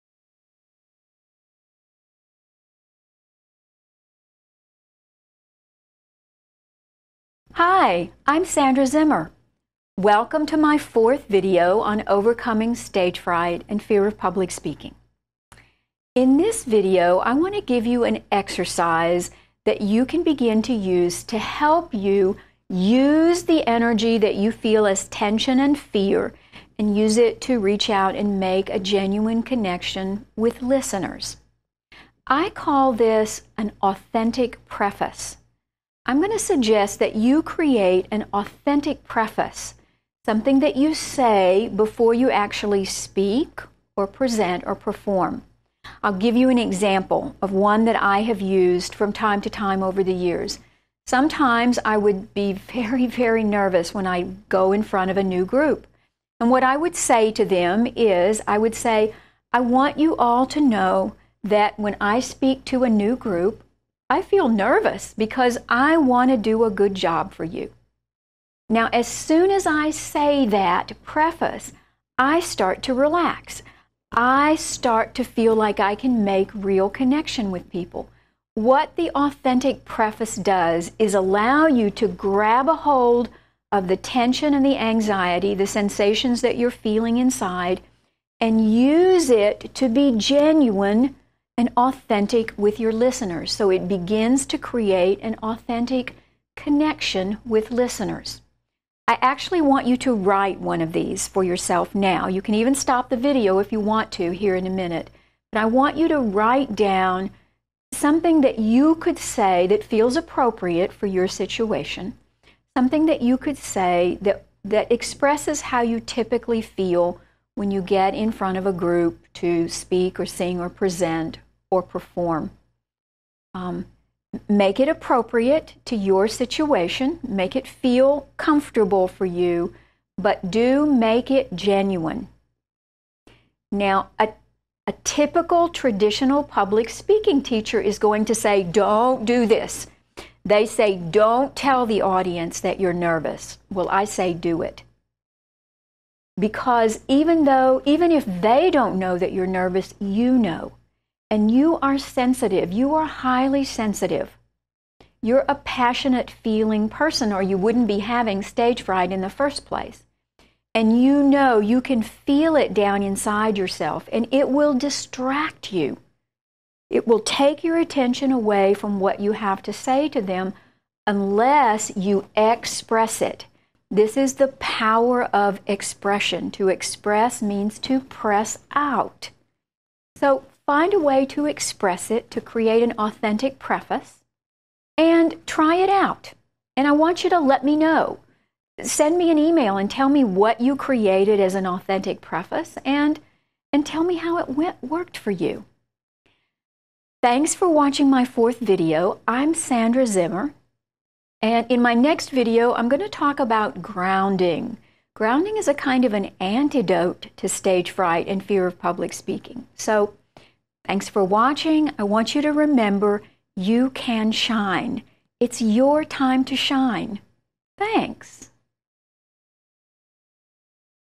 Use Fear of Public Speaking to Create Genuine Emotional Connection
Sandra Zimmer demonstrates how to use an authentic preface to your speech that frees you to be real with listeners and opens you to create a genuine connection with them while also improving your public speaking skills.